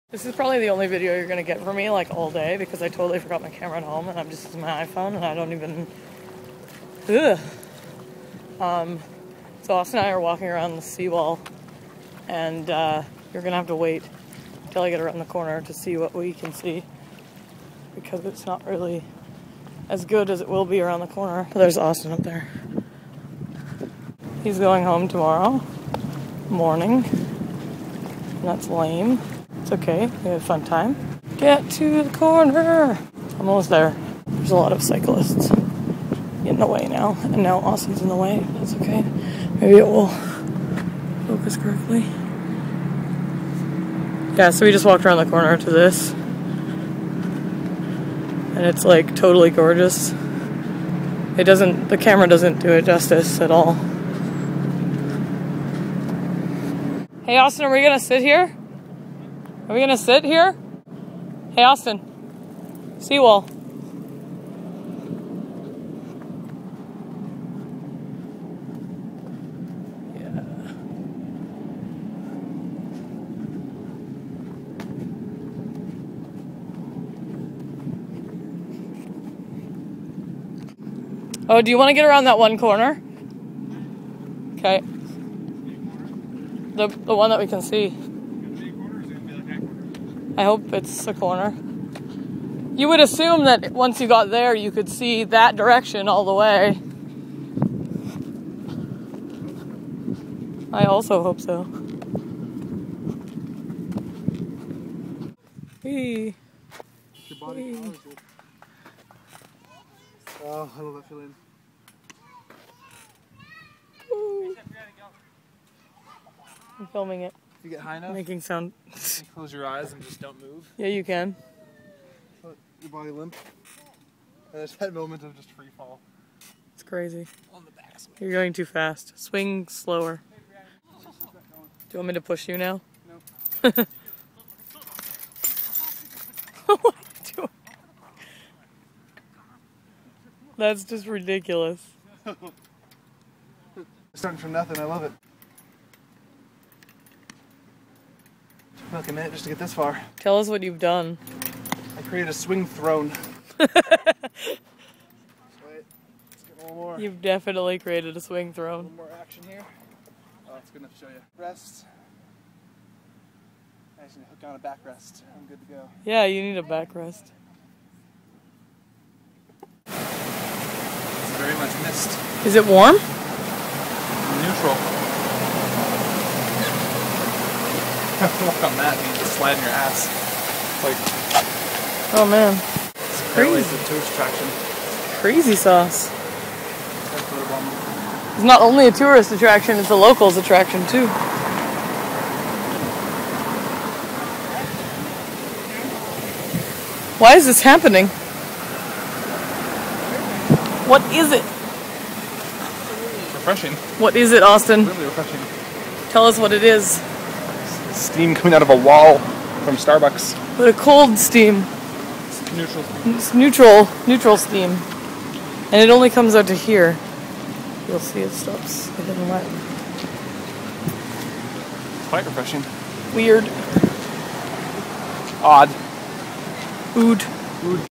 Hey! Seawall! [September 13th, 2011]
Austin and I PULL ourselves around Vancouver's Seawall.